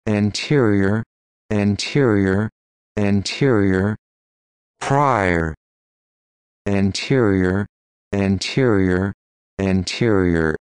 SAT Vocabulary Words and Definitions — Anterior
This video shows you how to say or pronounce Anterior and gives the definition of Anterior.

What's your definition of Anterior?